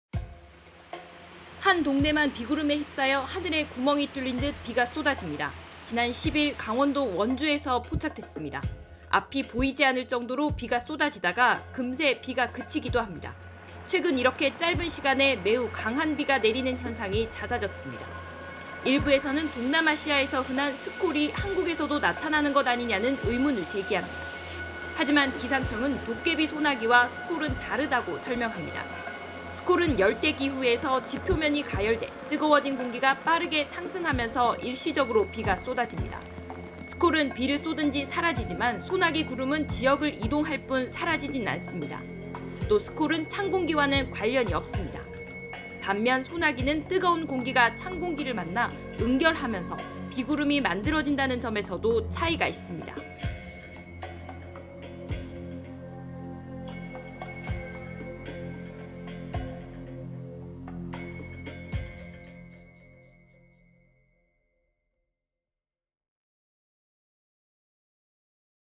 갑자기 쏟아지는 비, 스콜? 소나기? ｜지금 이 뉴스
최근 짧게 시간에 매우 강한 비가 쏟아지는 상황이 반복되고 있습니다. 일부에서 동남아시아의 '스콜'이 한국에서도 잦아진 게 아니냐는 의문을 제기합니다. 하지만 기상청은 '소나기' 현상이며 스콜과는 차이가 있다고 합니다. 그 차이는 무엇일까요.